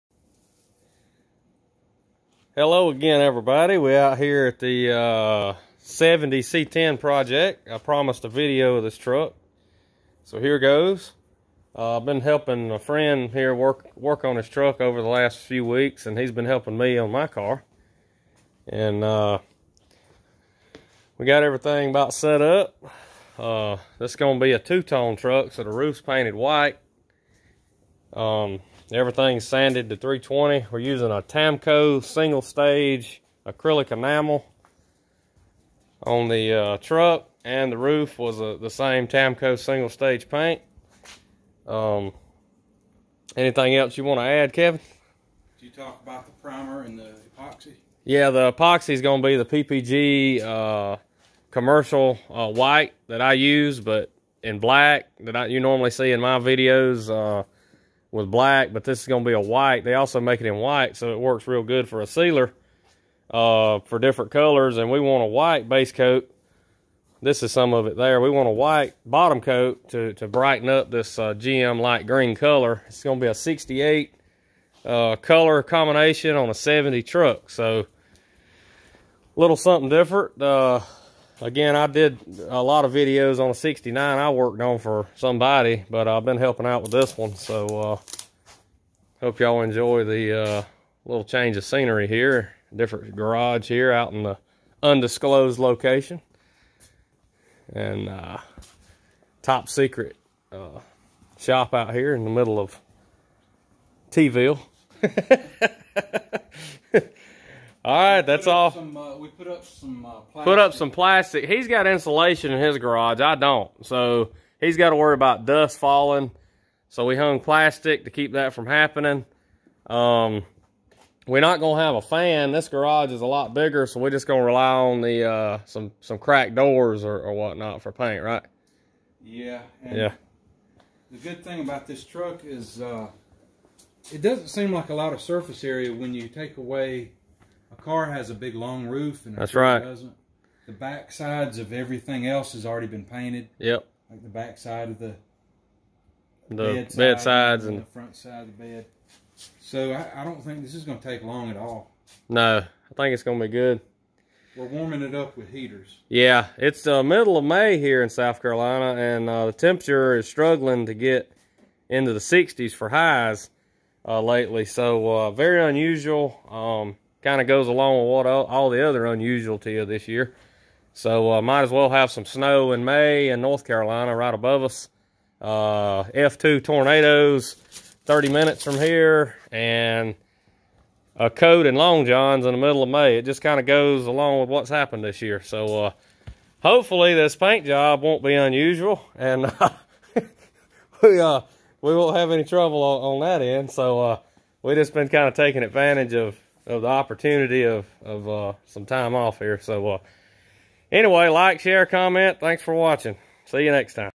1970 C10 Project
A truck I've been helping on pretty steady the last 2 months.
Painted today.